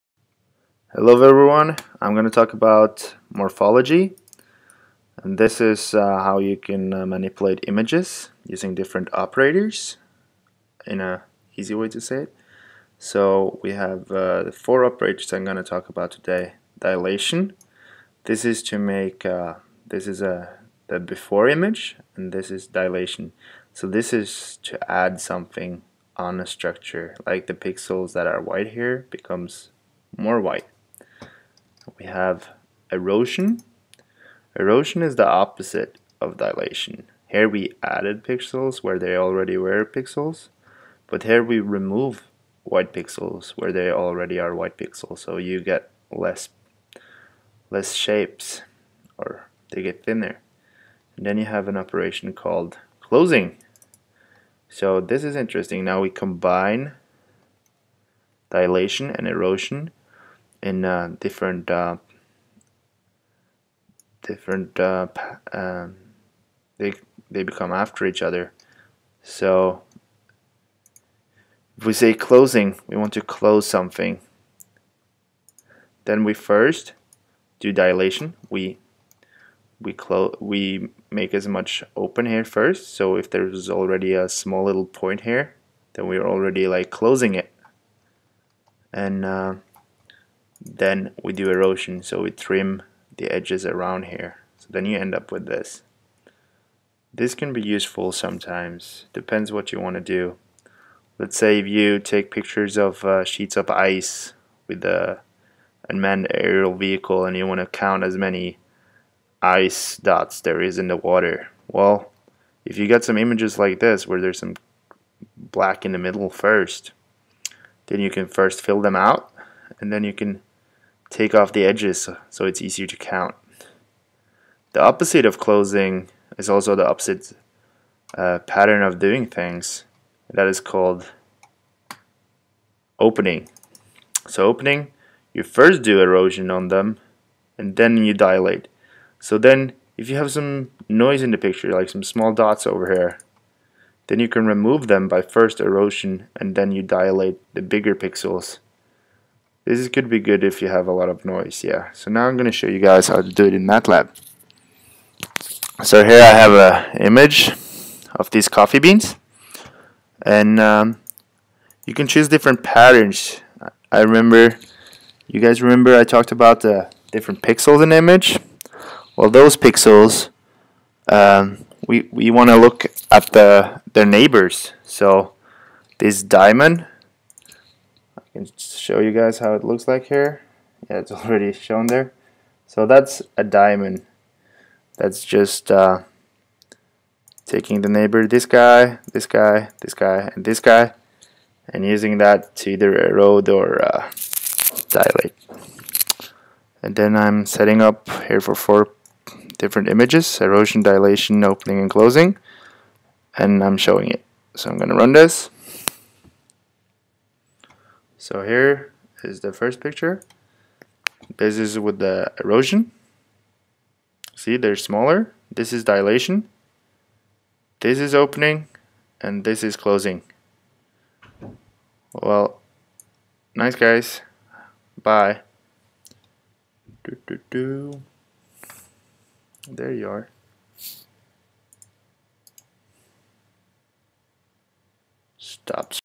Morphology- dilation,erosion,closing,opening
Short explanation of dilation,erosion,closing and opening.